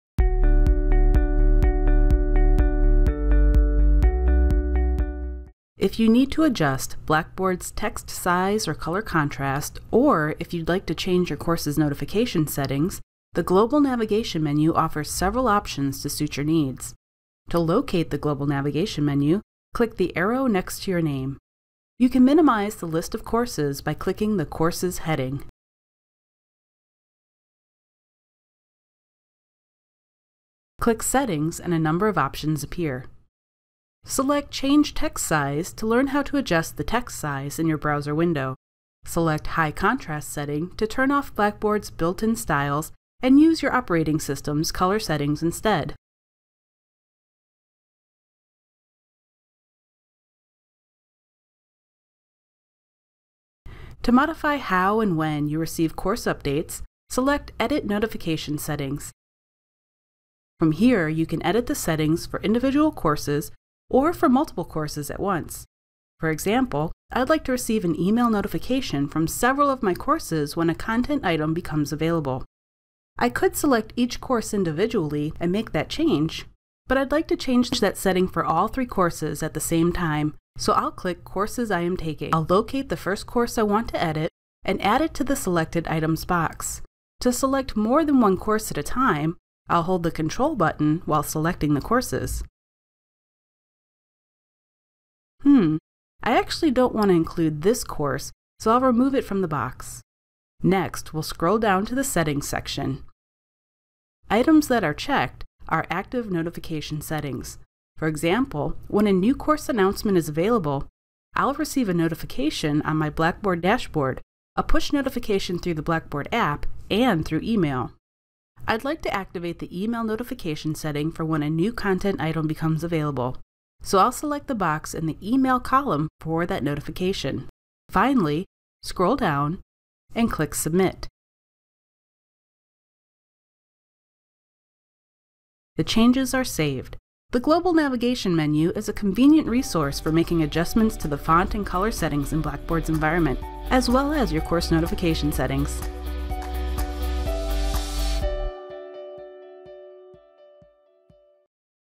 eLearning Intro: Using Global Navigation to Change Text Size, Contrast, and Notification Settings
This video demonstrates how to use Blackboard's Global Navigation Menu to change the text size, color contrast, and Course Notification settings in the Blackboard environment. This video is part of the eLearning Intro Video Series for new online students at Wake Technical Community College.